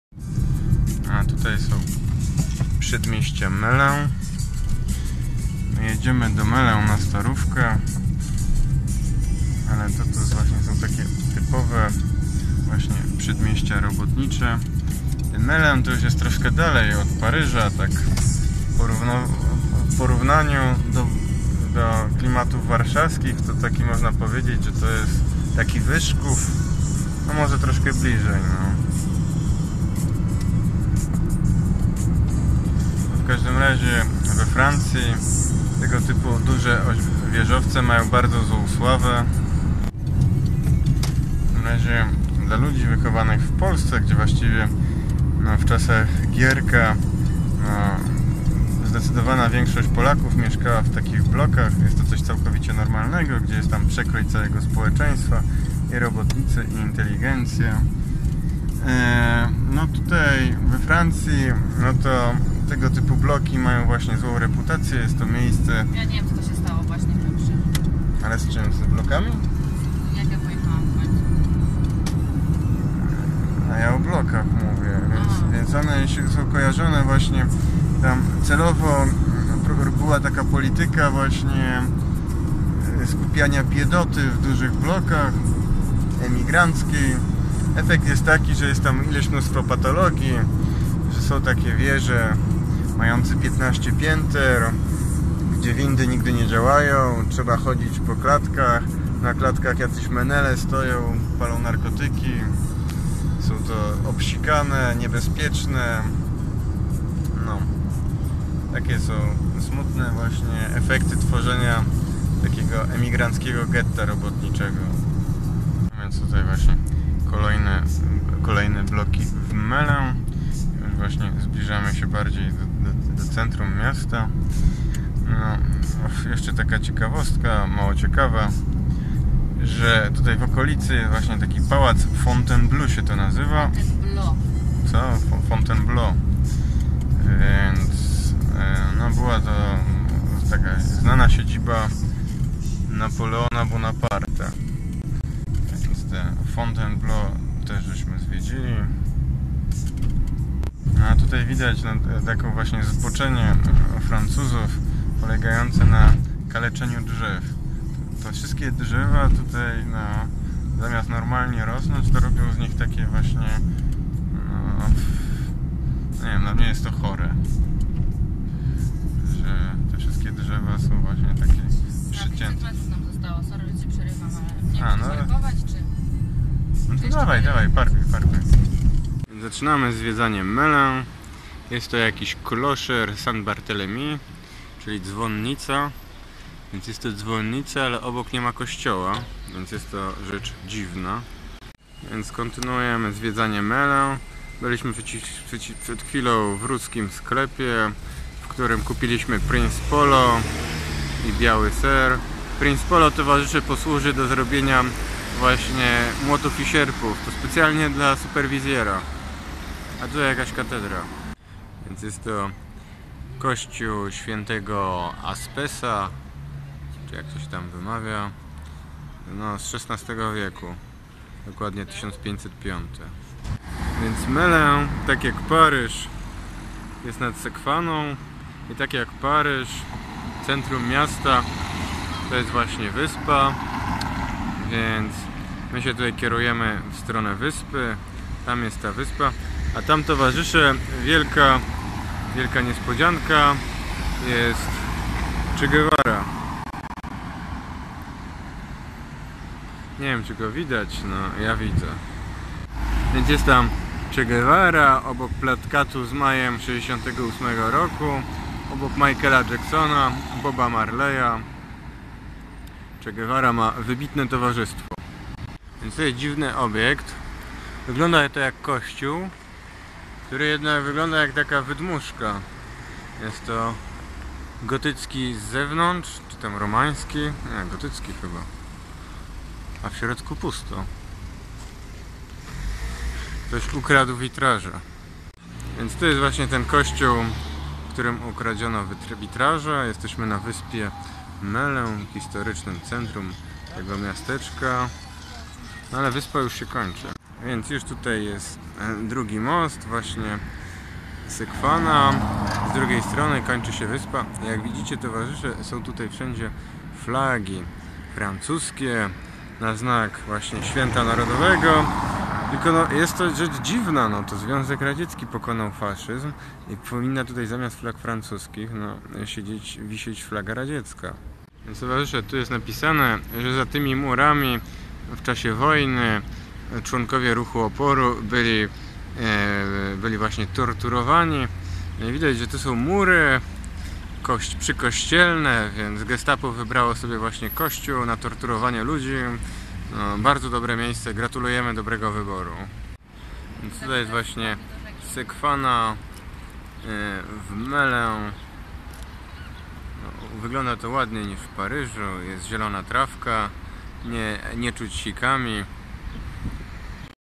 Zwiedzanie Melun - podparyskiego Wyszkowa
Melun to mała mieścina nad Sekwaną na południowy wschód od Paryża. Znana głównie z pętli RER D.